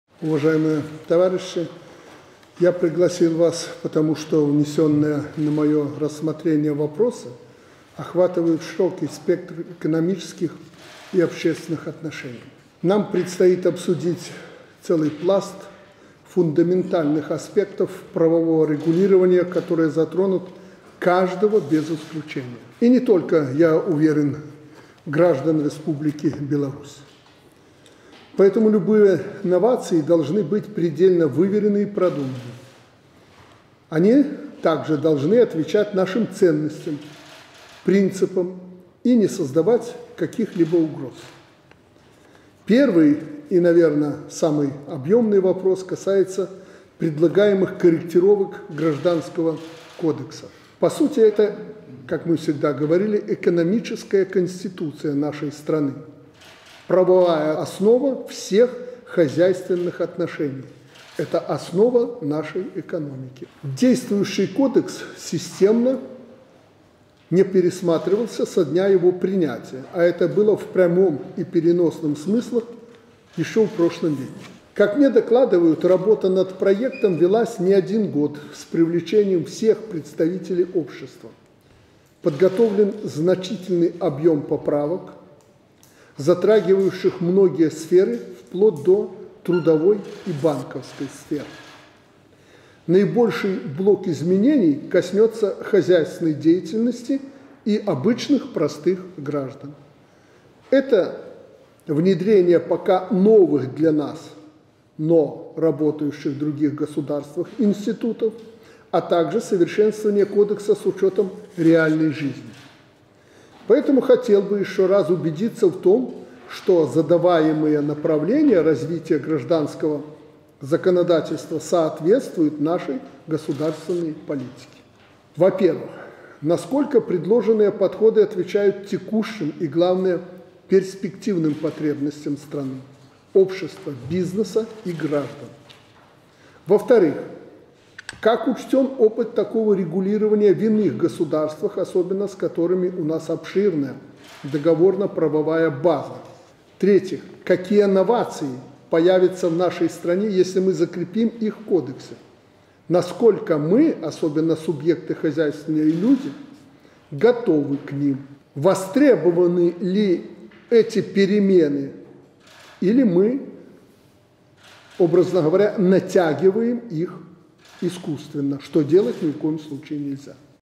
Лукашенко: Востребованы ли эти перемены или мы "натягиваем" их искусственно?
Лукашенко: Какие новации появятся в нашей стране, если мы закрепим их в кодексе? Насколько мы, особенно субъекты хозяйствования и люди, готовы к ним?

Александр Лукашенко собрал совещание по вопросам правового регулирования хозяйственной деятельности. Как отметил в начале совещания глава государства, вынесенные на рассмотрение вопросы охватывают широкий спектр экономических и общественных отношений. Первый и самый объемный вопрос касается корректировки Гражданского кодекса.